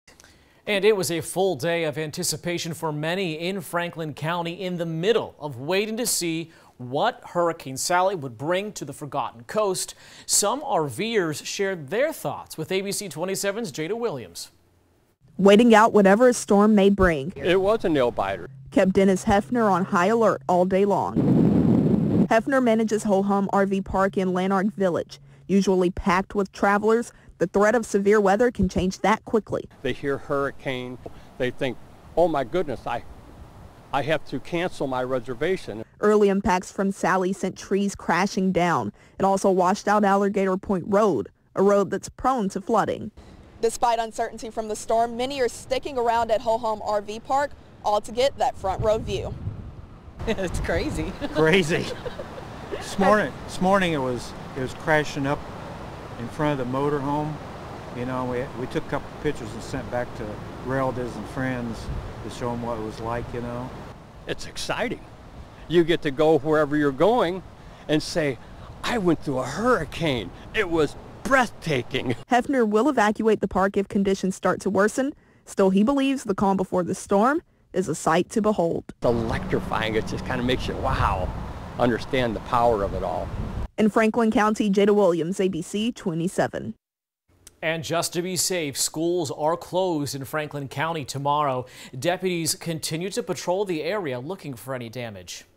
Franklin County RV park spared from worst of Hurricane Sally on Monday
Electrifying is how one Franklin County described the storm surge and waves felt in Lanark Village Monday.